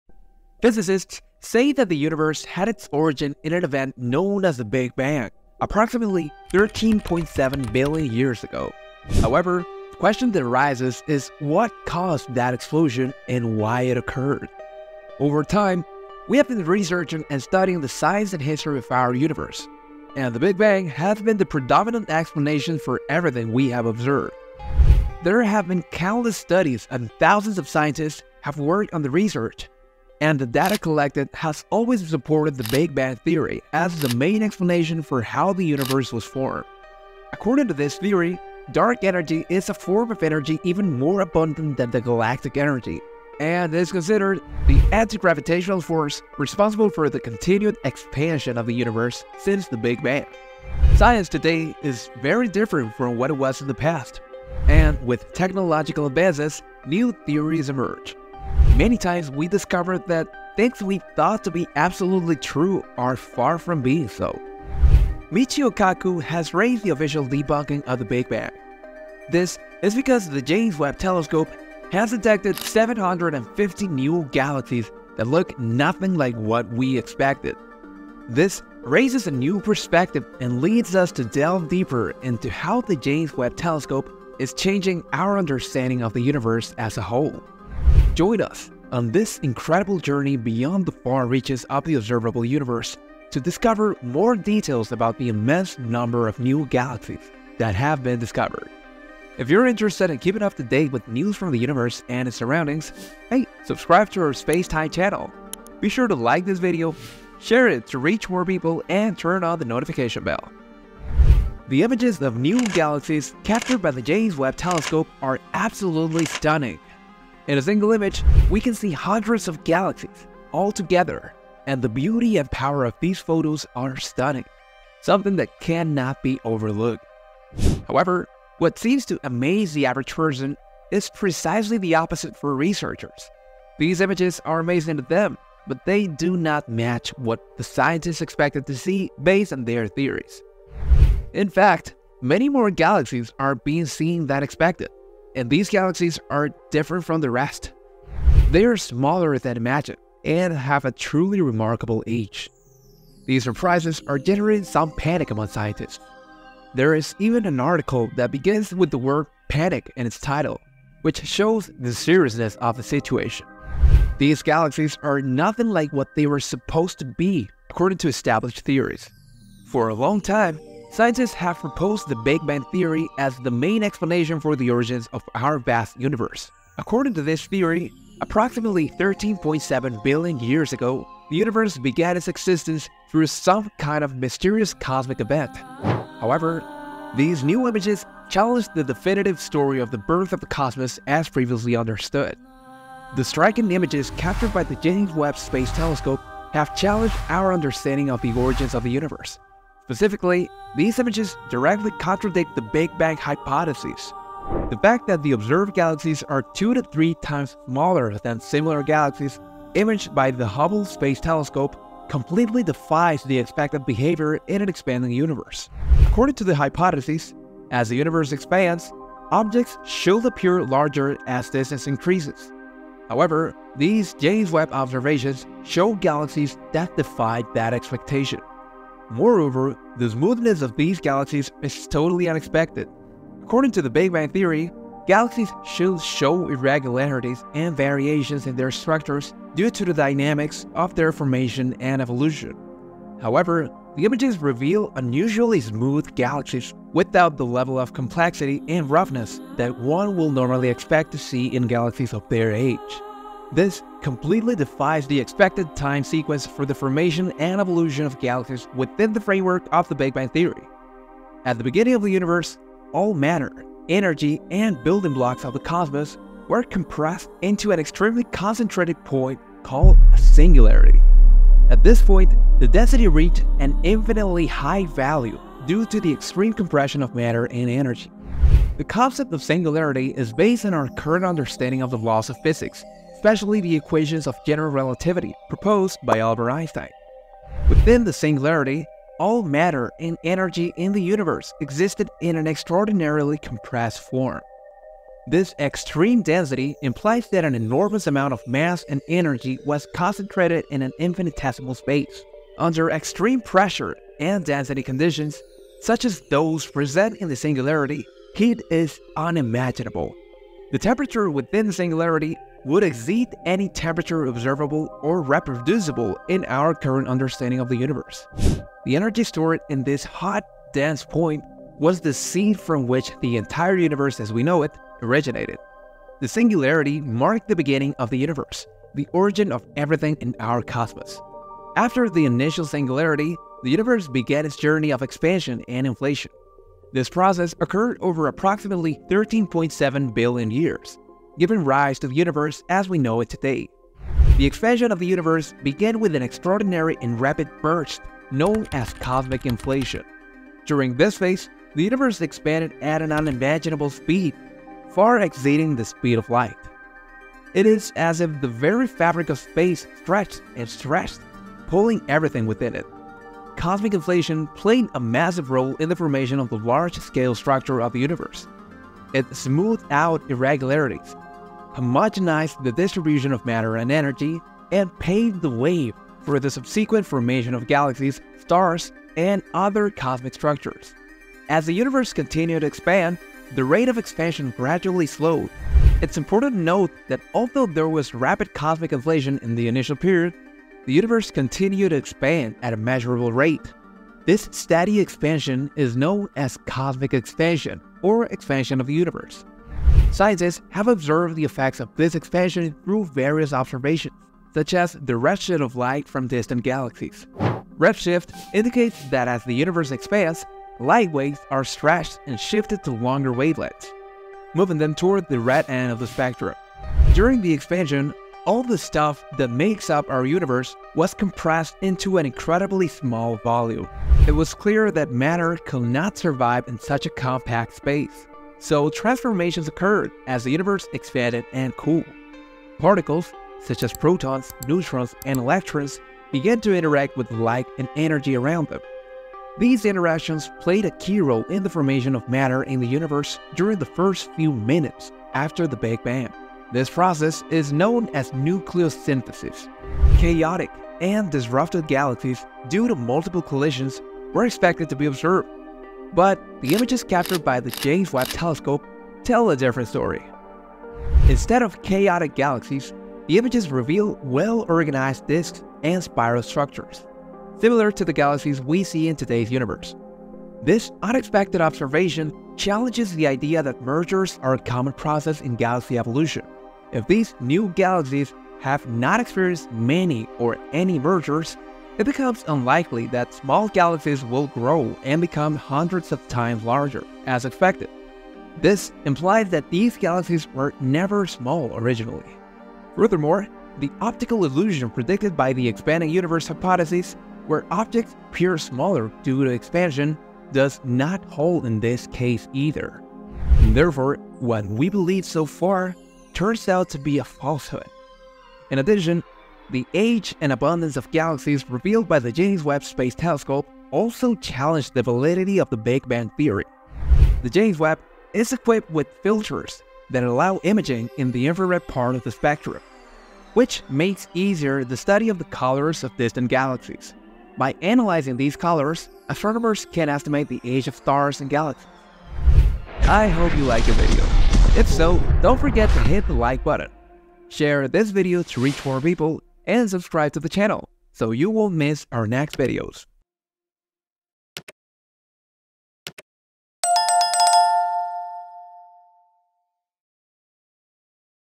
Neil deGrasse Tyson: “James Webb Reveals 750 Galaxies Beyond the Observable Universe”
Neil deGrasse Tyson's shocking revelation has shaken the astronomical field: the James Webb Space Telescope has detected a staggering 750 galaxies beyond the observable universe. This extraordinary announcement has captured the imagination of the scientific community and generated profound awe among space enthusiasts.

The discovery of these additional galaxies reveals a vast, previously unknown cosmic landscape, challenging our existing perceptions of the universe. The James Webb Telescope's ability to go beyond the observable significantly expands our understanding of the vastness of the cosmos.

This finding provides a unique window to explore distant regions of space, offering crucial data for understanding the evolution and complexity of our universe. The detection of these galaxies beyond what was thought to be achievable highlights the importance of advanced technology in the continued expansion of our knowledge of cosmic mysteries.